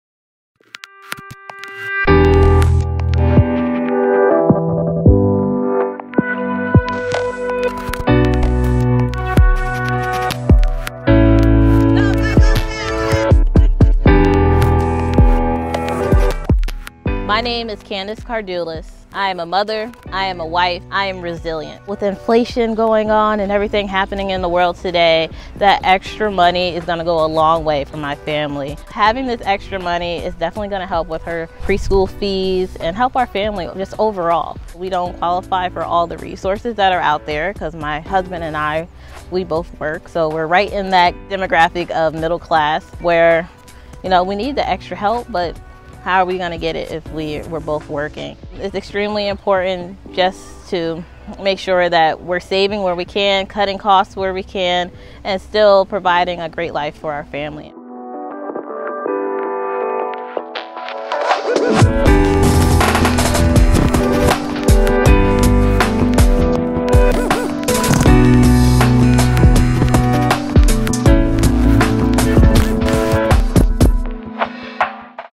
Create a PERMANENT CT Child Tax Credit NOW!
NOW is the time for Connecticut to join with the rest of the nation to support families with the high cost of raising children. Call or email your legislator today to advocate for a permanent $600/child for up to three children